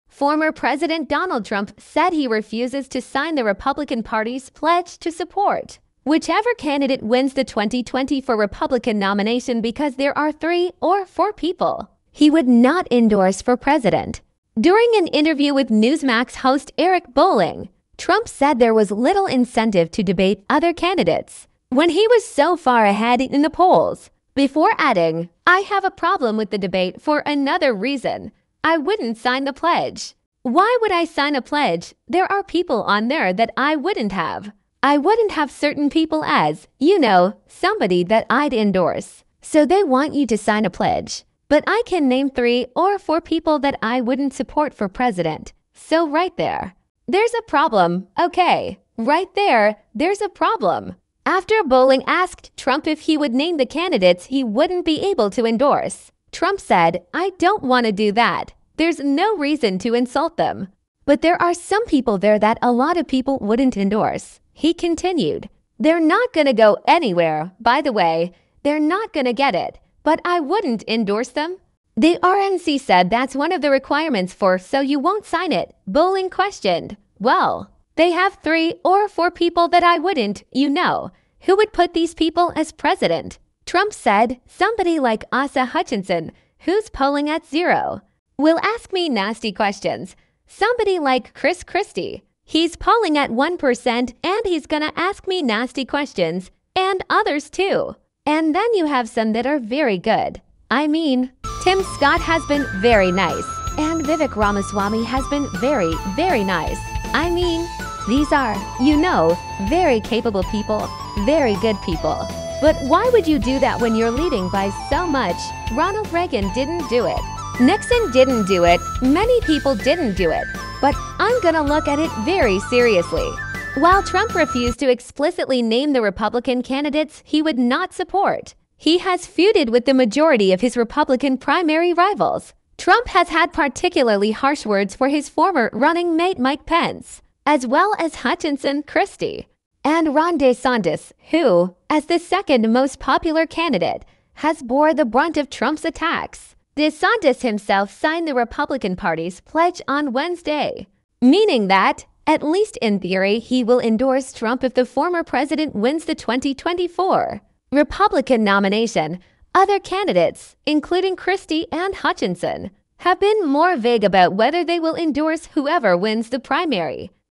Trump Refuses GOP Pledge over Opposition to Certain Nominee Candidates
Trump Says He Won’t Sign GOP Pledge Because There’s ‘Three or Four People That I Wouldn’t Support’ If They Won the Nomination
Trump, GOP, presidential nomination, political pledge, candidate support